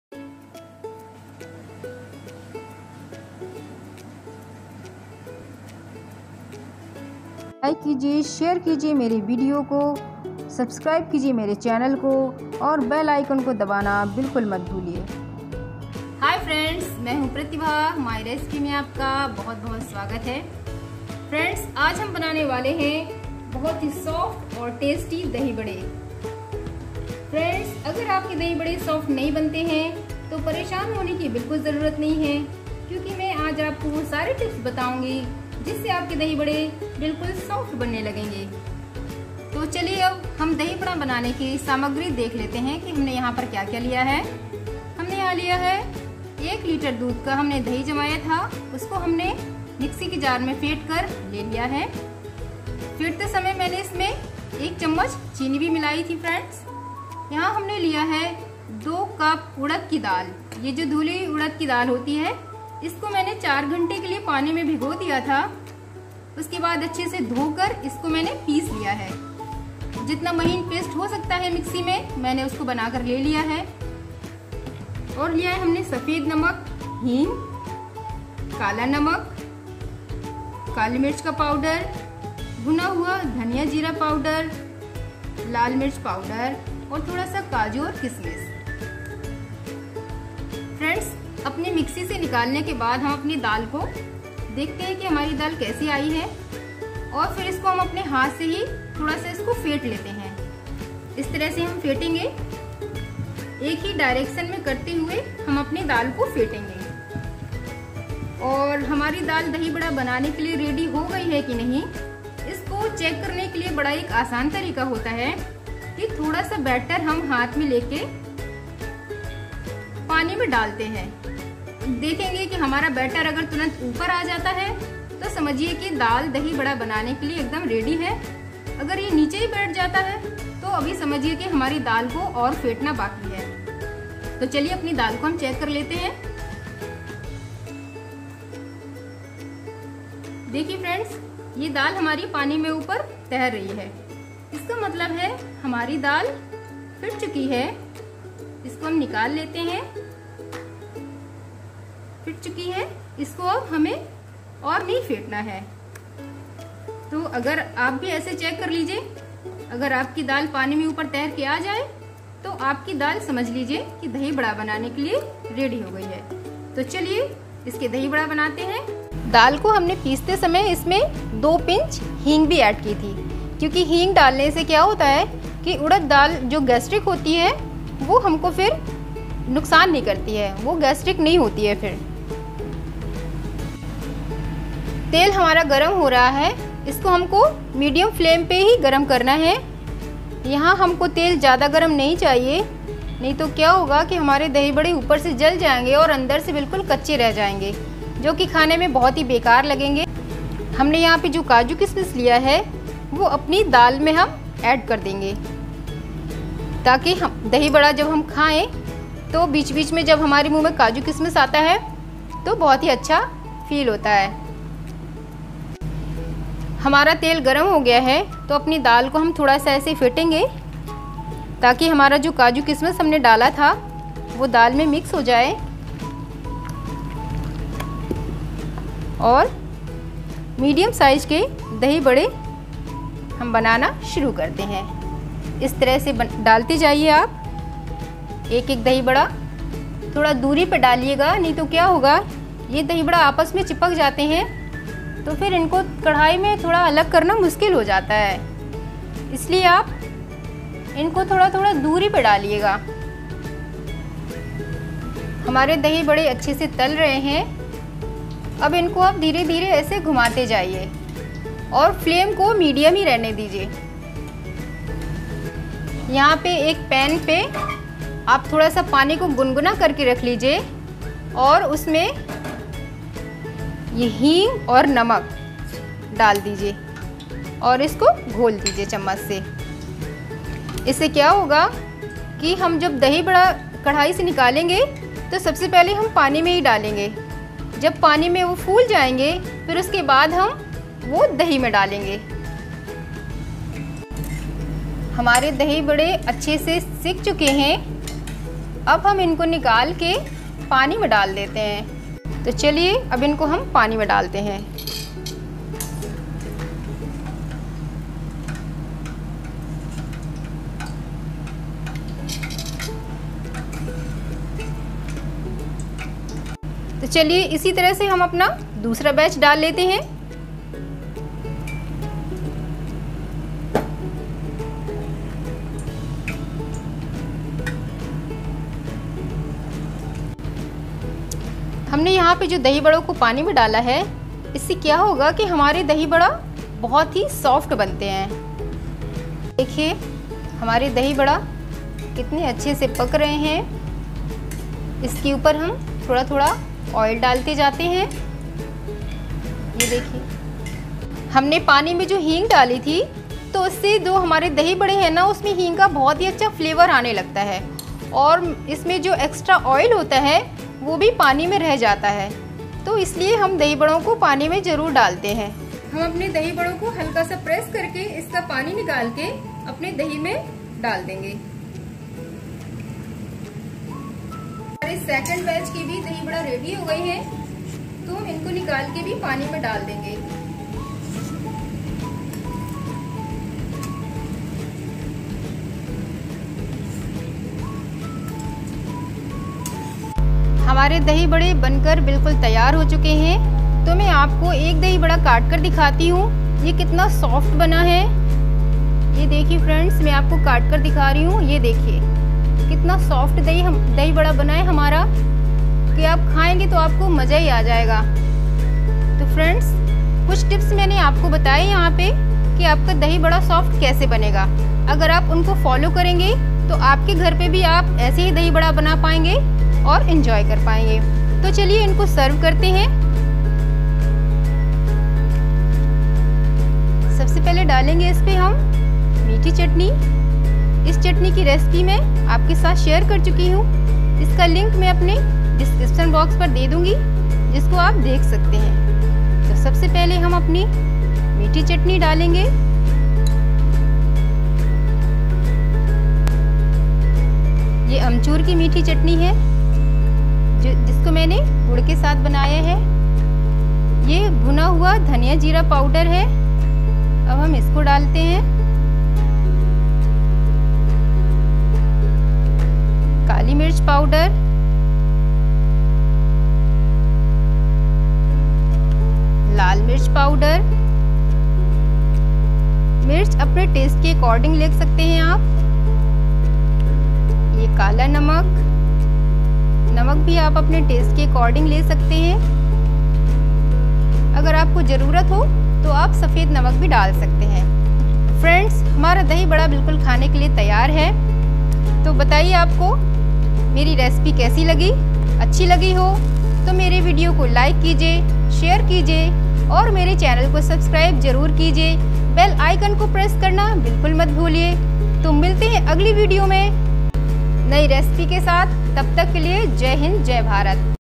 dahi bhalle recipe/dahi bhalle/dahi bade recipe/dahi bade banane ki vidhisoft dahi bade kaise banaye
Aaj hum banana vale hai soft aur tasty dahi bhalle (Dahi vade)
List of ingredients
1.Dhuli urad dal – 2 cup
2.Dahi – 1 kg
3.Hing – 2 pinch
4.Sugar – 2 tea spoon
5.Salt – according to taste
6.Black salt – ½ tea spoon
7.Bhuna hua dhaniya jeera powder – 2 tea spoon
8.Red chilli powder – 1 tea spoon
9.Refine oil – ½ litre
10.Khatti mithi chutney 1 cup
11.Cashew (kaju) – 1 table spoon
12.Rasins (kishmish) – 10-15 pcs
13.Black pepper powder – 1 tea spoon